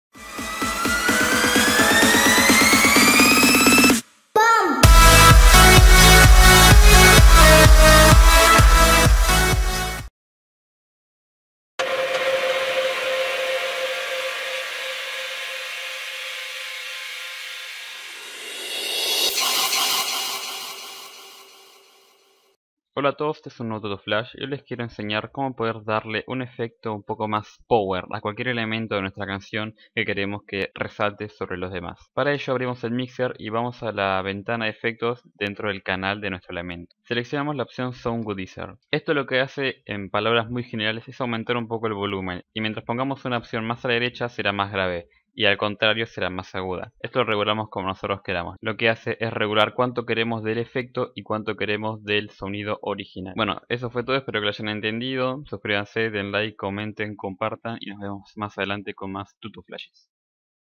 Cómo usar el Soundgodizer | TutoFlash12
Nunca fuimos tan puntuales en esta sección, 1min justo, ni más ni menoooosss! Espero que lo valga :D!

Me costó mucho subir este vídeo ya que no me encuentro/encontraba en una zona con mucha señal de Internet (digamos que estoy "robando wifi", como dije en el último vlog), pero igualmente aquí lo tienen :D! Pero hablando del vídeo en sí, aquí les enseñaré a utilizar en "Soundgodizer", una herramienta del FLStudio muy útil a la hora de mezclar.

Deja tu like y comenta así puedo saber tus opiniones/consejos!

____________MIS REDES___________